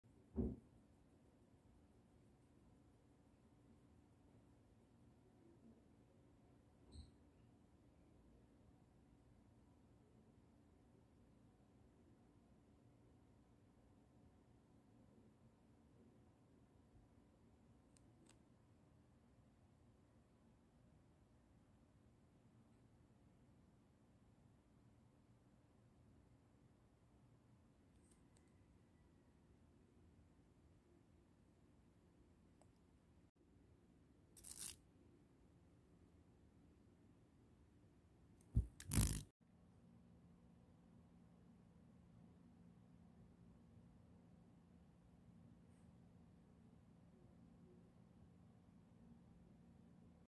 DRAGONFLY IN MY ROOM
Isang Gabi nakita ko siya sa looob nang room ko  Pero kinabukasan nagpaalam din siya😭